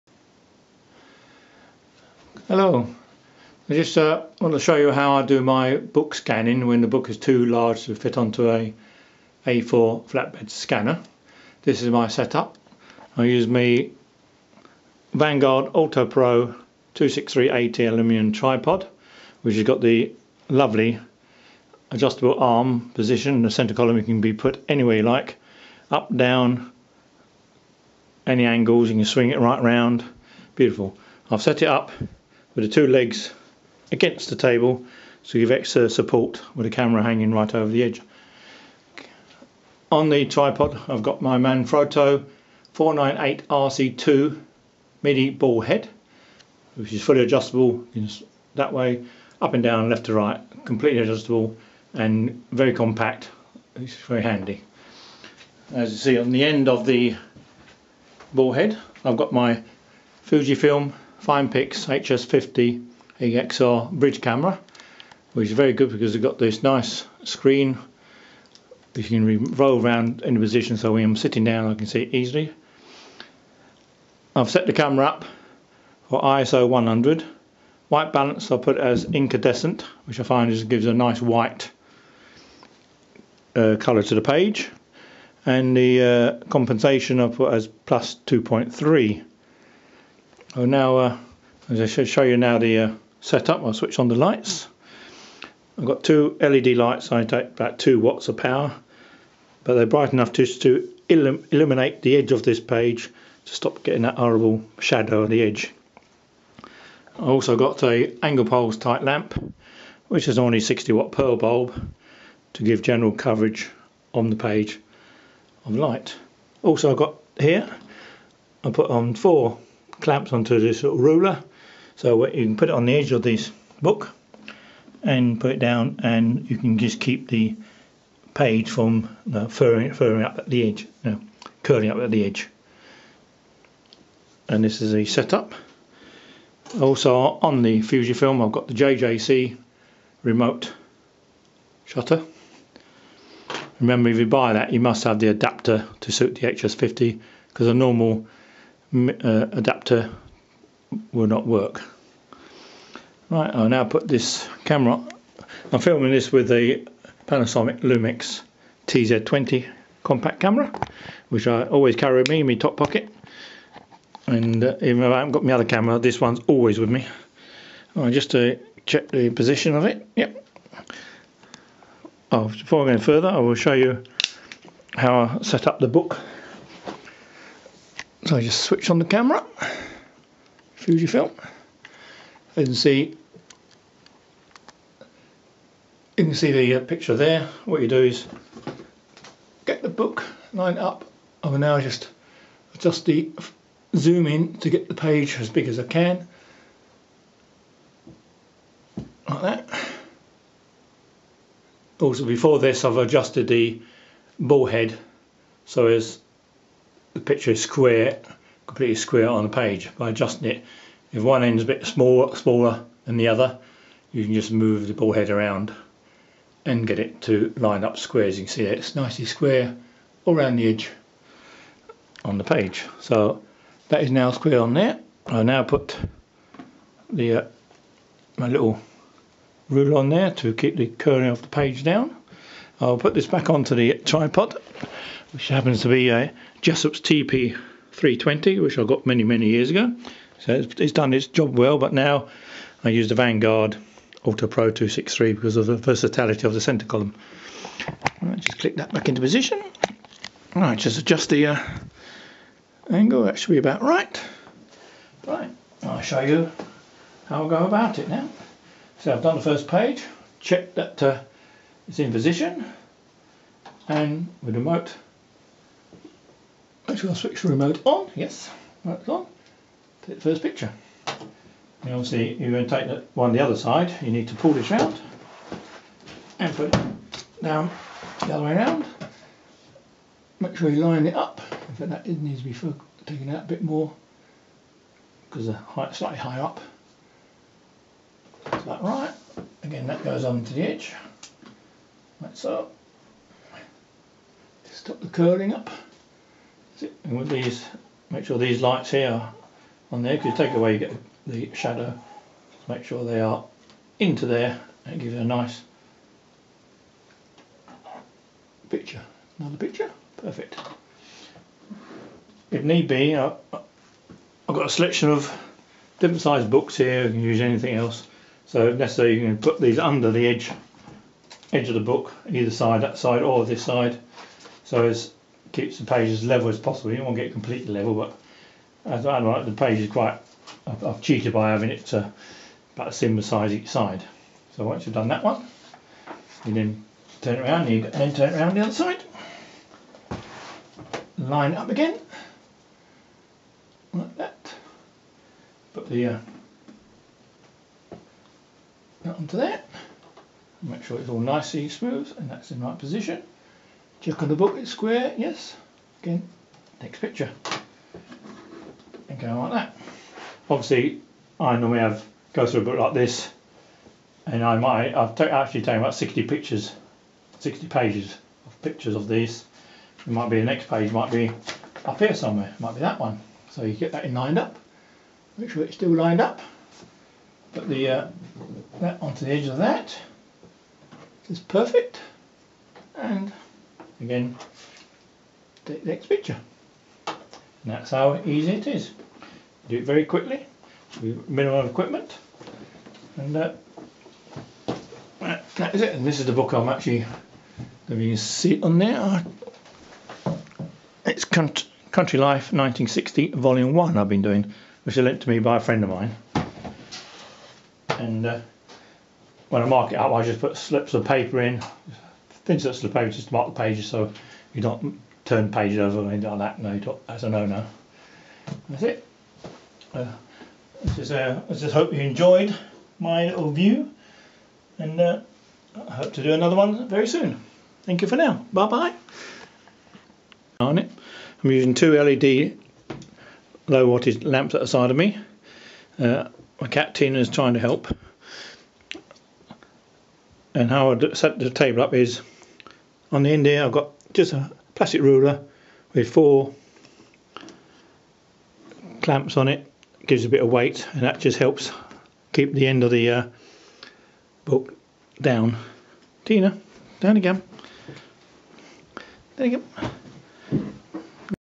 Book Scanning using Vangaurd AltaPro 263AT Tripod
How I scan large books with my Vangaurd AltaPro 263AT Tripod and Fujifilm Finepix HS50 Bridge Camera